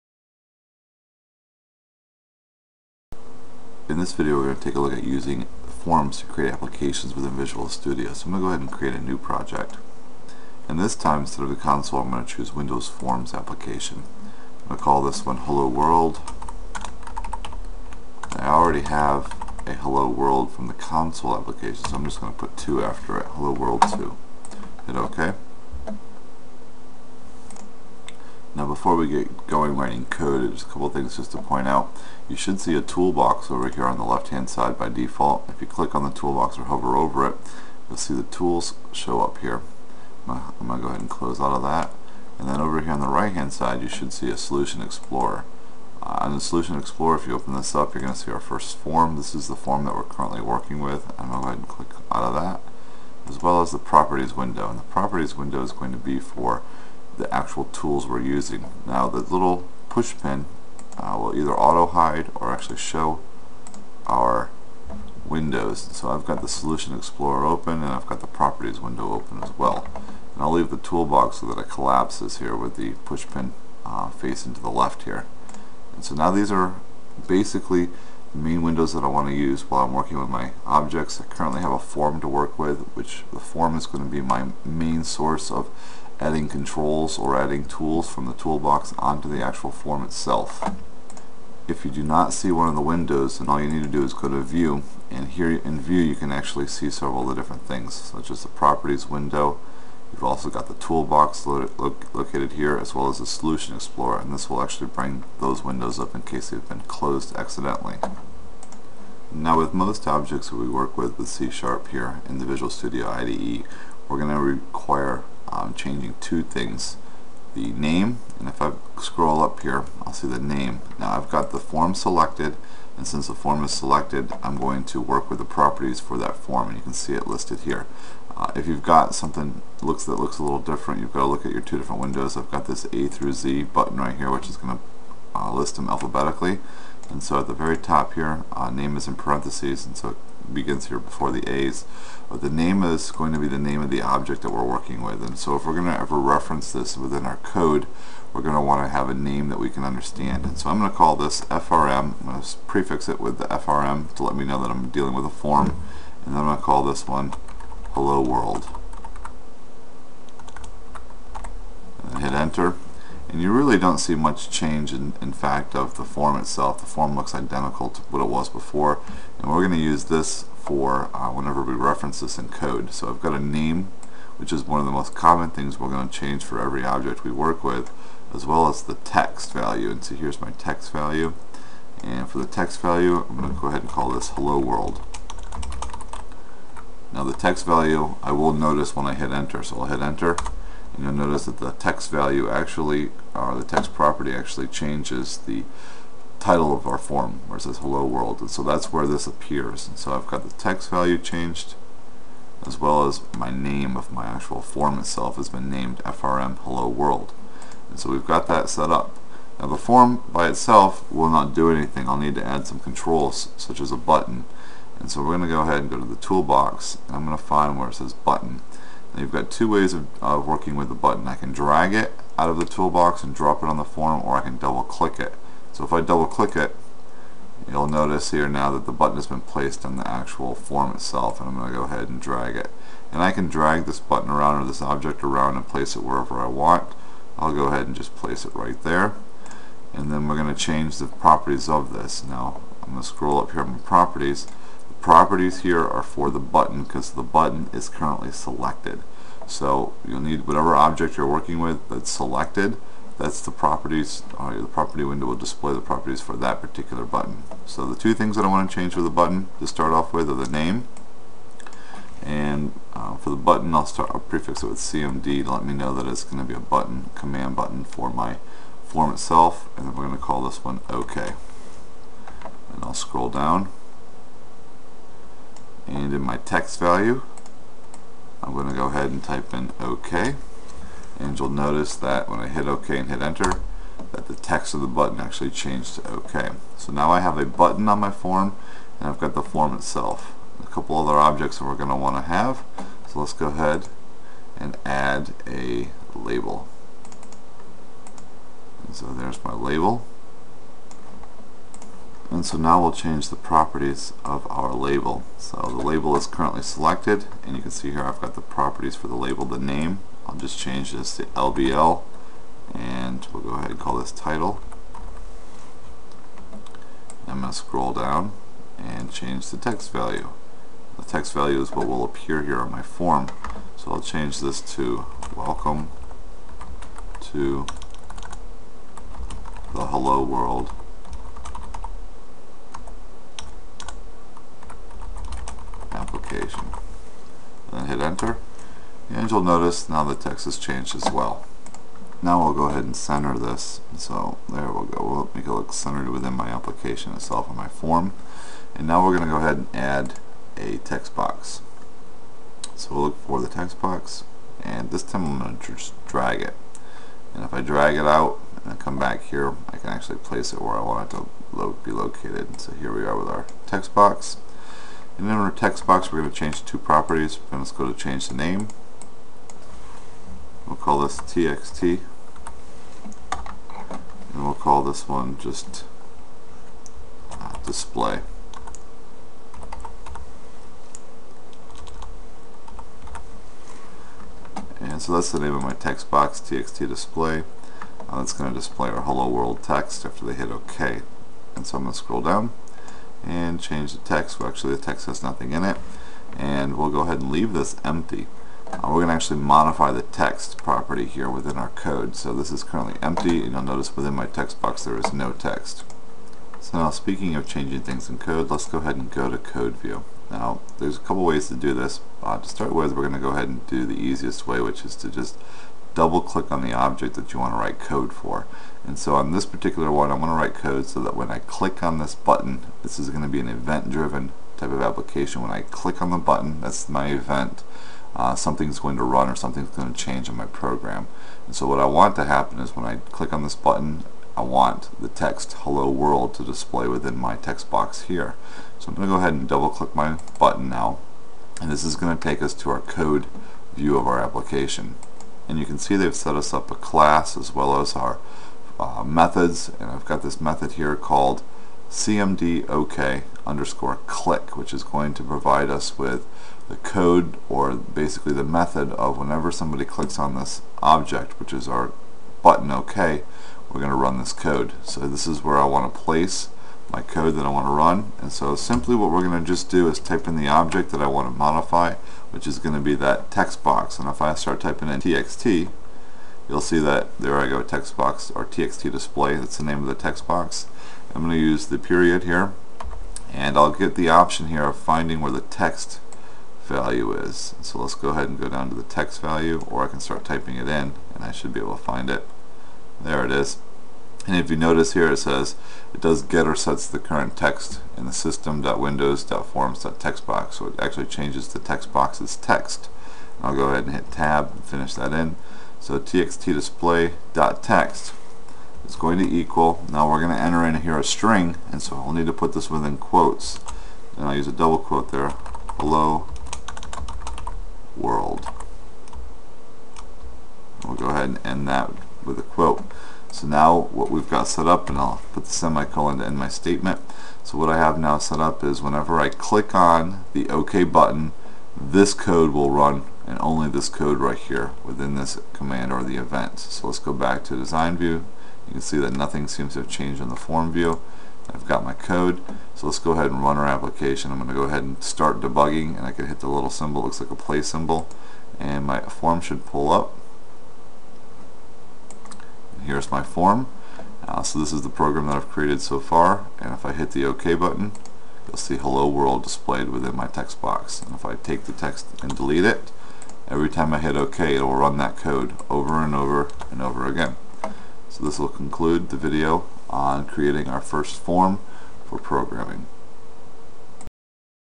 Visual C# 2010 Lesson 10 - Introduction To Creating Forms
This video is part of a video series on the C# programming language using Visual Studio 2010.